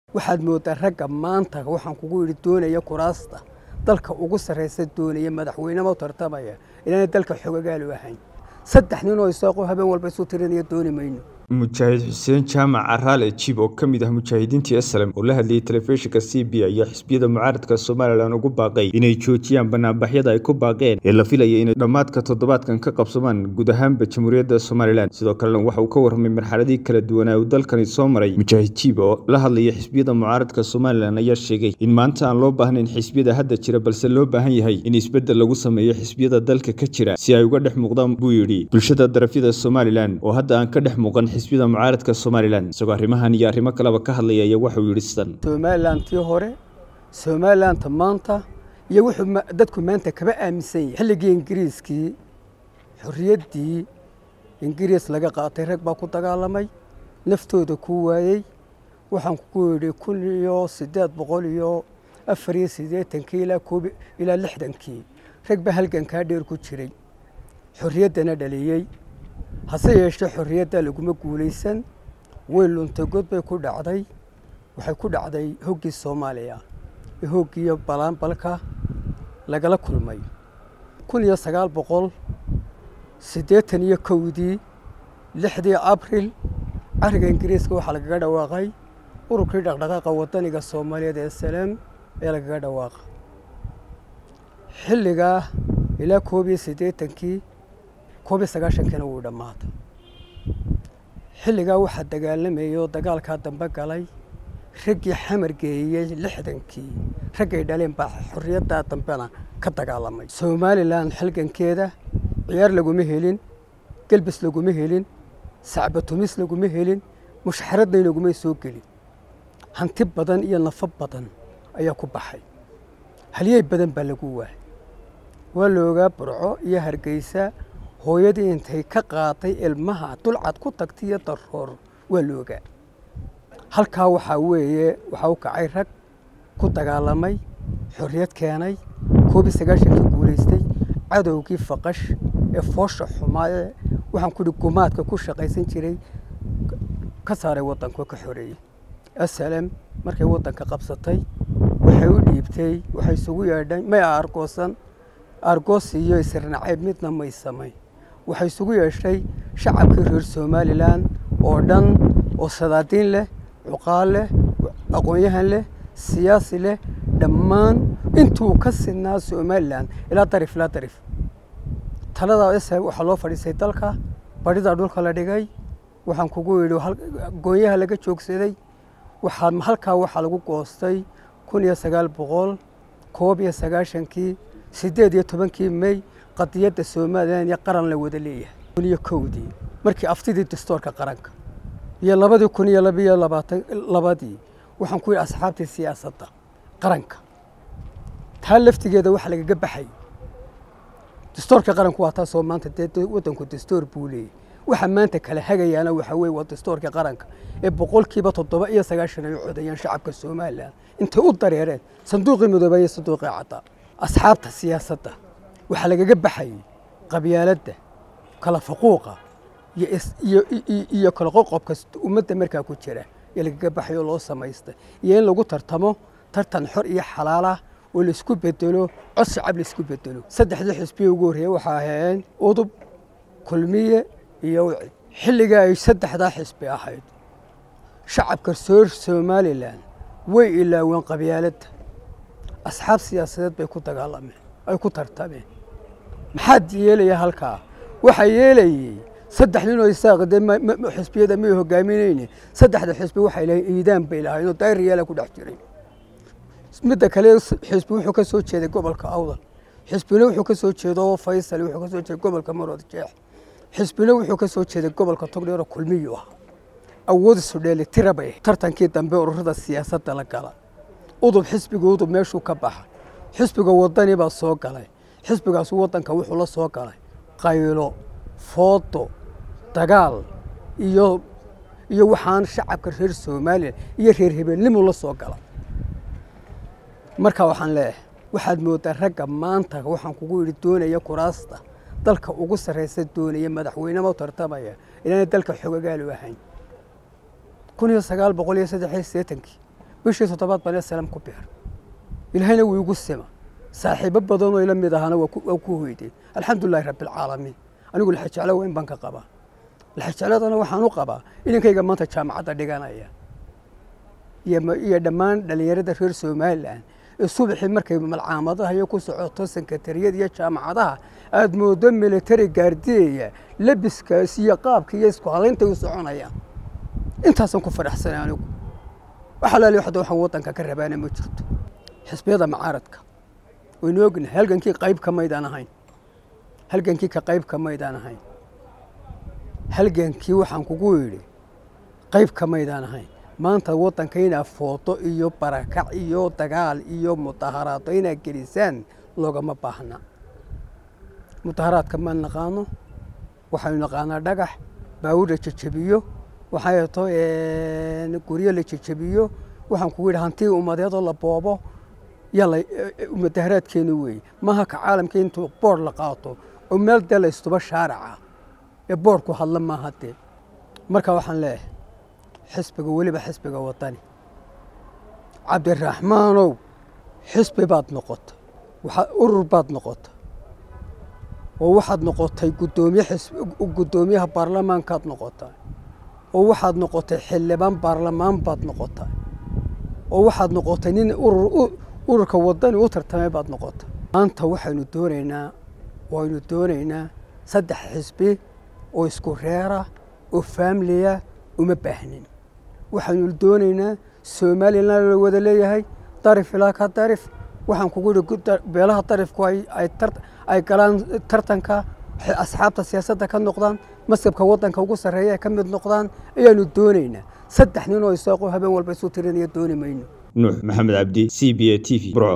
Halgamaa Xuseen Jiib ayaa ka hadlay Banaanbaxyada Mucaaridka Somaliland
"Waxaad Moodaa in Ragga Maanta Kuraasta Dalka ugu Saraysa ku Tartamayaa in aanay Dalka Xogogaal u aheyn" Mujaahid Xuseen Jiib